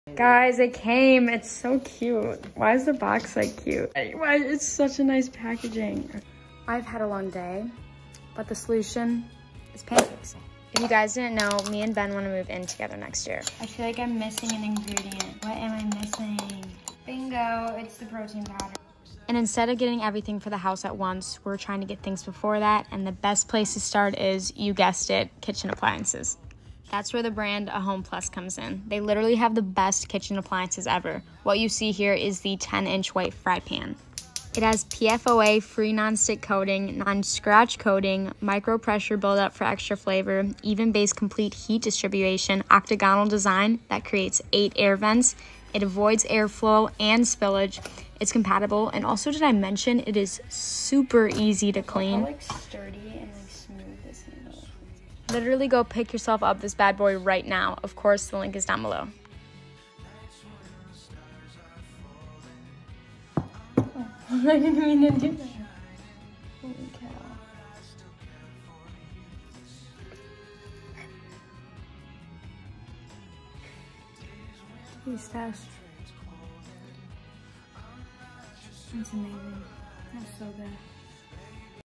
PEEK8 Premium Non-stick Fry Pan Unboxing & Delicious Pancakes Recipe by Ava Yuergens
Thanks, Ava Yuergens for sharing  PEEK8 Premium Non-Stick Cookware on your channel.

(copy and paste into google)
►Ava Yuergens discount:

Banana Dark Cholocate Pancakes Recipe;
1 banana
1 egg
1/2 cup almond milk
3 tablespoon oats
1 tablespoon protein powder
1/2 cup baking powder